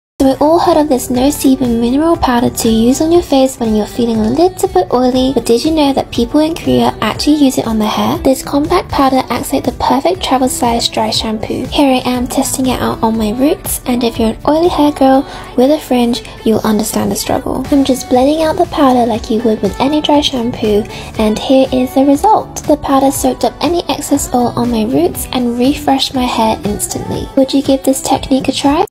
Oily Hair Hack Trending in Korea with INNISFREE No Sebum Powder
This best-selling powder is great for oily skin, but did you know it’s actually also used for mattifying oily hair?😍 Agnes is here to show how Korean It Girls use this multifunctional star!

🤍INNISFREE No Sebum Mineral Powder (5g): One of the most loved Korean beauty products of all time – this translucent loose mineral powder has double sebum-control properties that immediately blurs pores and leaves your skin matte, fresh and shine-free! The star ingredients in this product are Jeju minerals and mint that controls oil production. The size of this powder means it’s perfect to carry around for touch ups.

Have you tried out this hack yet?🙌